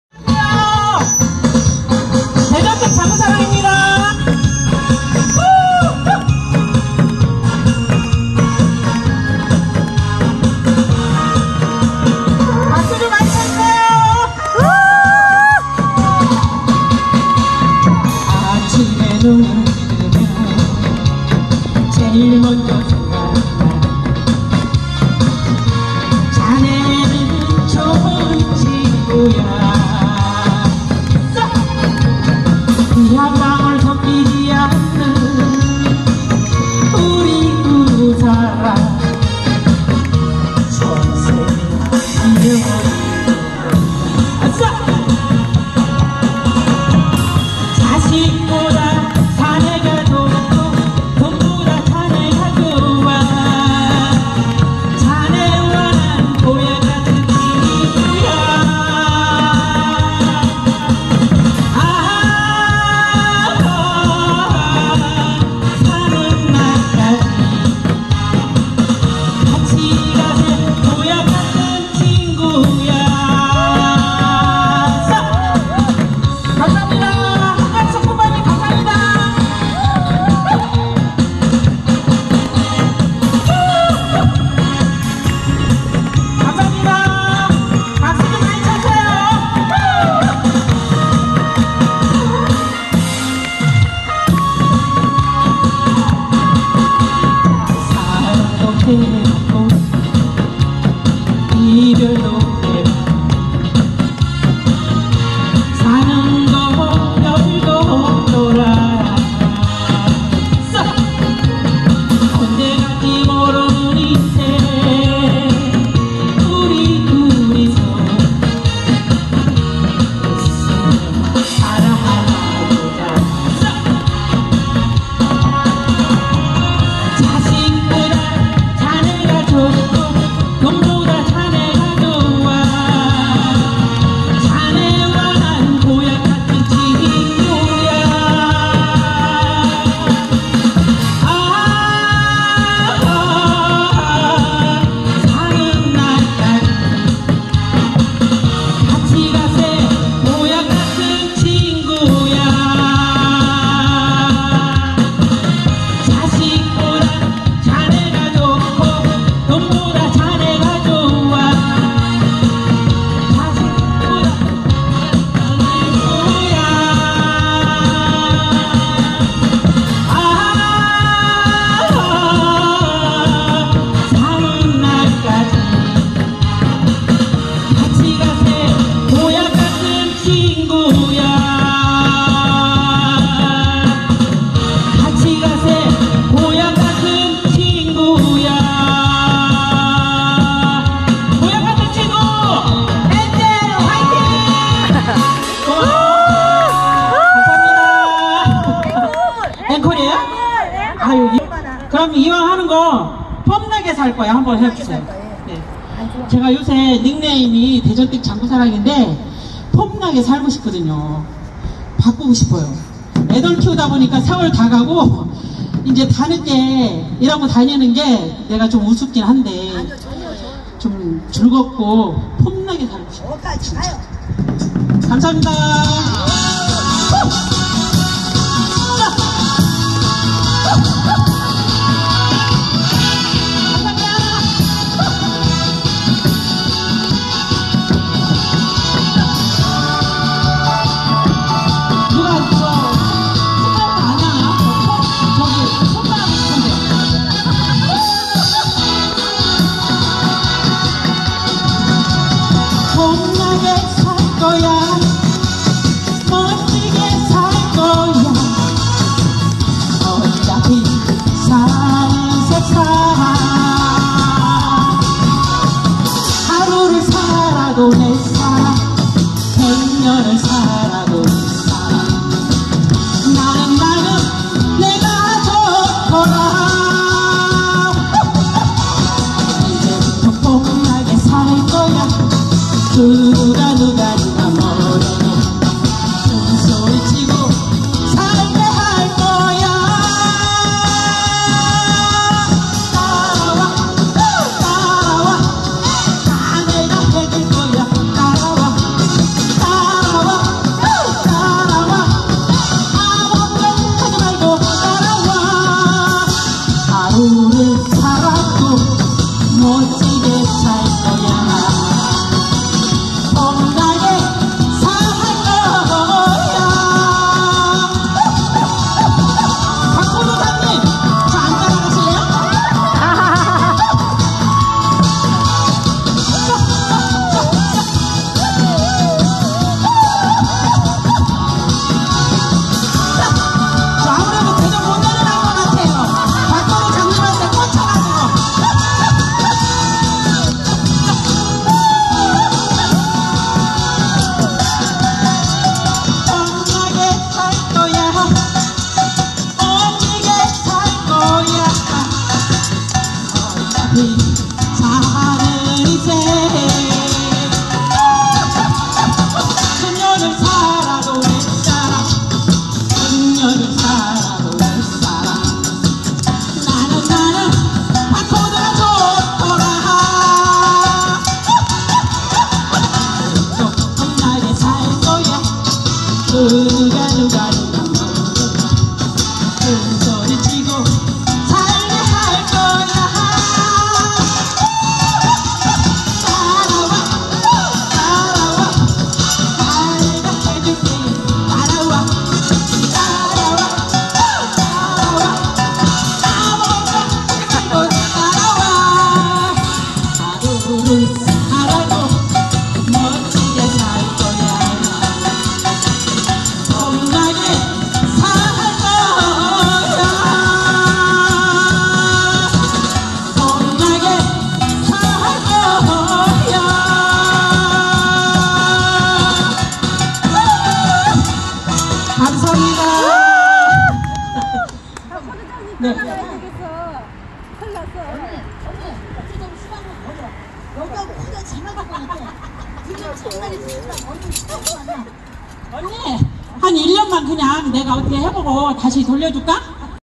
한강수품바공연장서😄품바하겟다고#들이대는대전댁😄바코드장품바님😄우짤까?대전댁 따라갈겨?
감독님께서 한곡조뽑으라고하셔서 영상을담게됏다 ‥
한강수품바님께 품바섭외하라고 들이대는 대전댁 ‥ㅎ
정말😄대전댁 품바할겨😄
바코드장님께서 😍코드를맞춰줘서 한껏
노래하기가 즐거웟던하루~^^